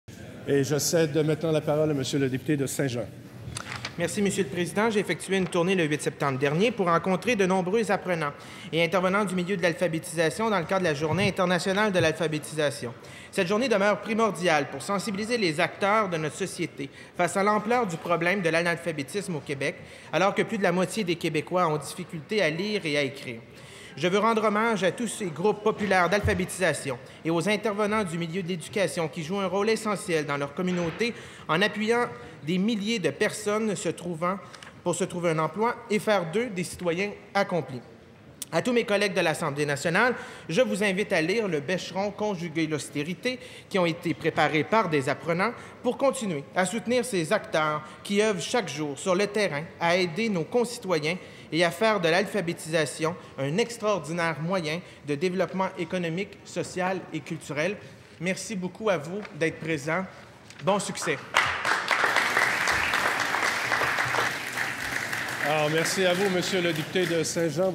Dave Turcotte souligne la Journée internationale de l'alphabétisation
– Le député de Saint-Jean à l’Assemblée nationale et porte-parole de l’opposition officielle en matière d’Alphabétisation, Dave Turcotte, a souligné le 15 septembre dernier, la Journée internationale de l’alphabétisation.  Une motion, présentée conjointement avec la députée de Saint-Marie-Saint-Jacques, Manon Massé, fut également adoptée à l’unanimité par les membres de l’Assemblée nationale. Cette motion se lit comme suit

Que l'Assemblée nationale reconnaisse l'ampleur du problème de
l'analphabétisme au Québec et de ses conséquences ainsi que ses liens
indéniables avec la pauvreté et l'exclusion;

Que l'Assemblée nationale reconnaisse la nécessité d'adopter des mesures
structurantes pour lutter contre l'analphabétisme au Québec;

Que l'Assemblée nationale reconnaisse le rôle essentiel joué par les groupes
populaires d'alphabétisation dans leur communauté pour, d'une part, contrer
l'analphabétisme et, d'autre part, pour appuyer des milliers d'individus dans
leur cheminement vers l'écrit et l'amélioration de leurs conditions de vie.